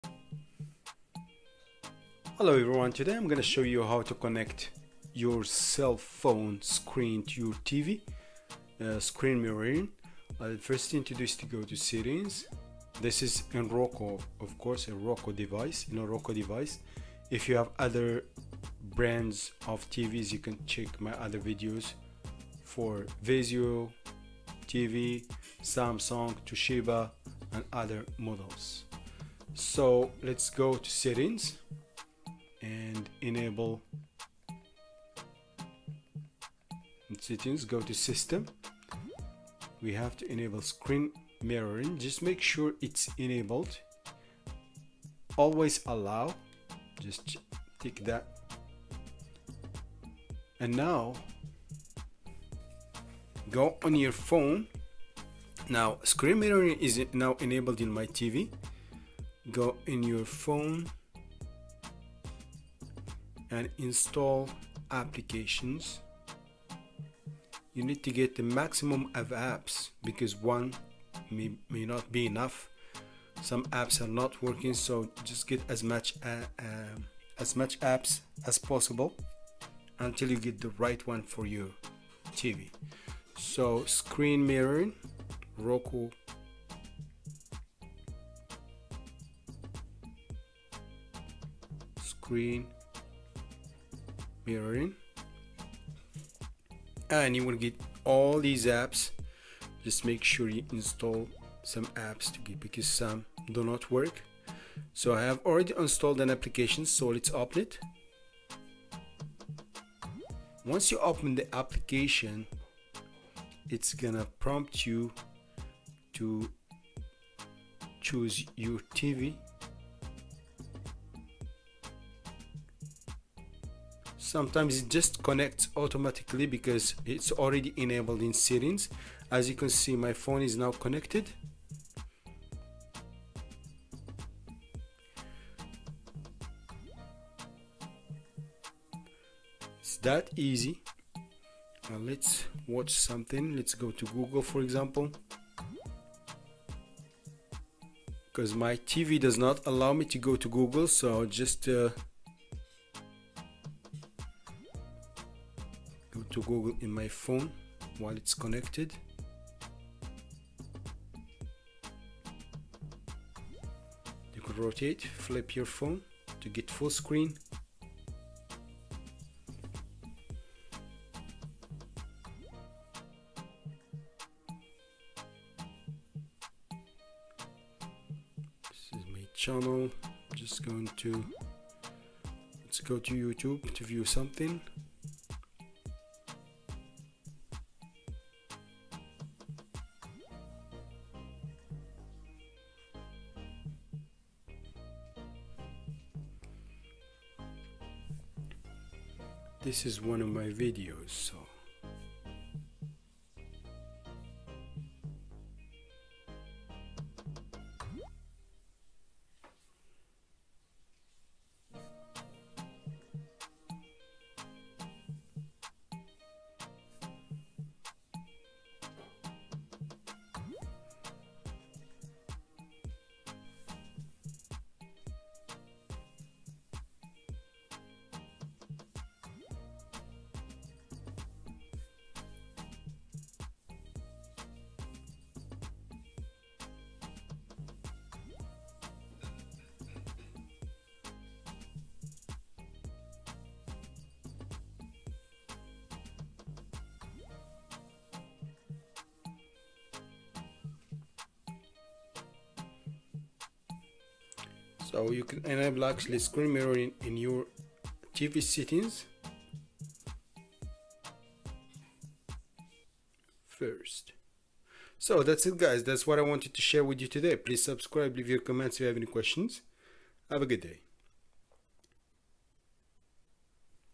How to Cast to Roku From Your Smartphone | Screen Mirroring
In this video, I am going to show you how to cast your phone to a Roku tv or Roku streaming device. To share a smartphone screen with a Roku device, you must first connect your phone and Roku to the same Wi-Fi connection.